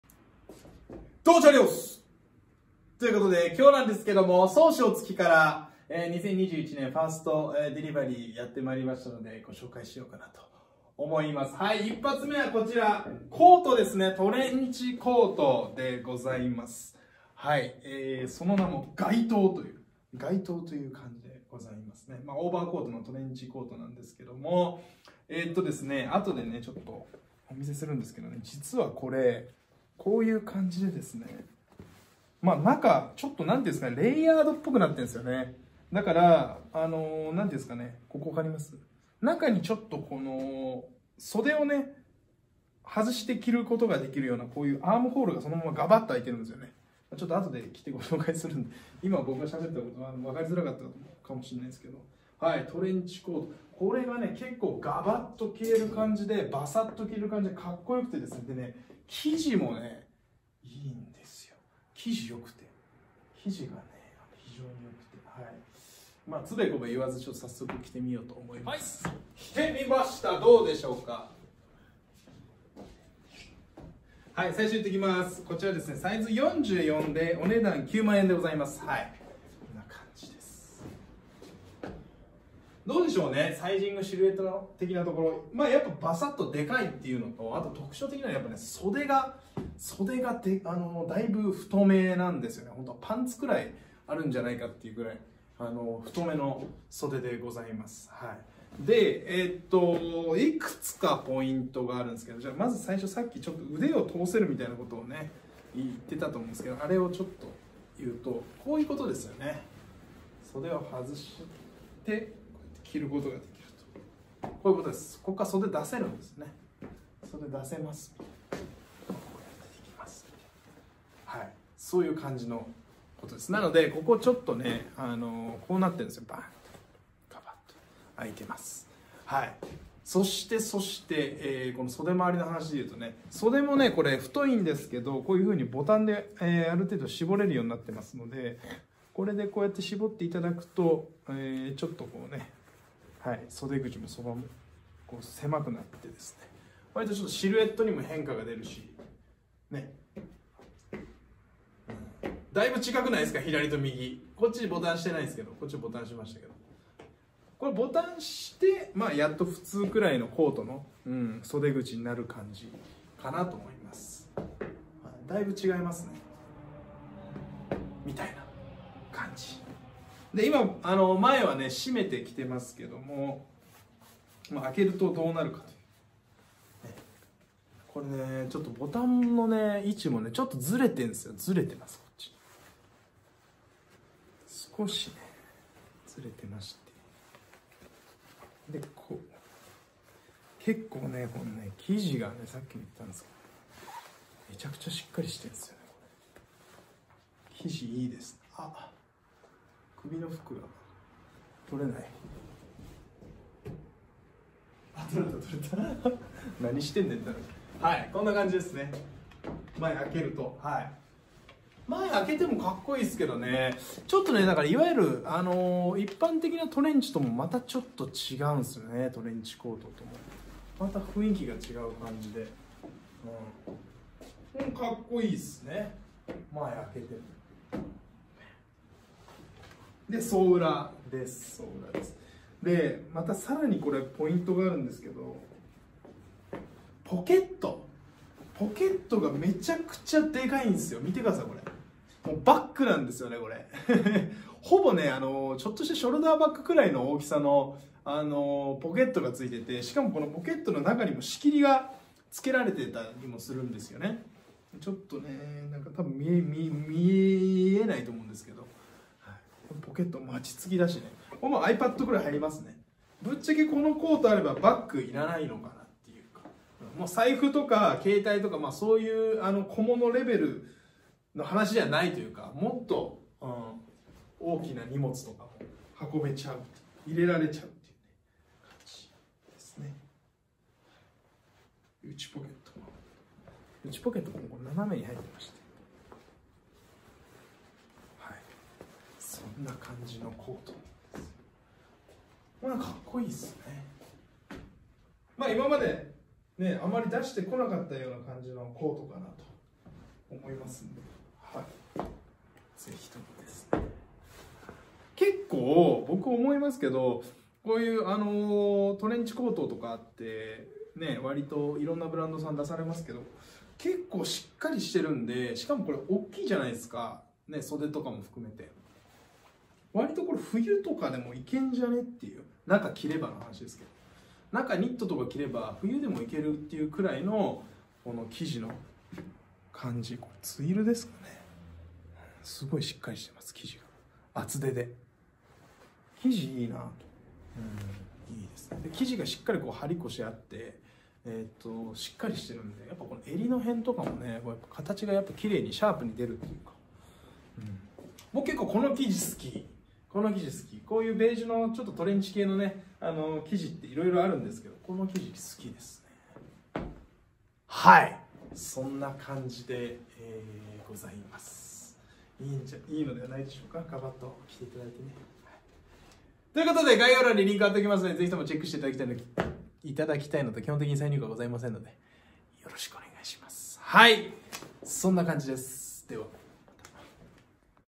【最速】SOSHIOTSUKI 21SS 1st！まずはコート！バサッと羽織ってかっこいい。生地もいいんすよ〜
詳細はこちら！※基本的に再入荷はございません
サブチャンネルはこちら！

ついでにアマゾン欲しいものリストはこちら！

ついでにブログもよろしく！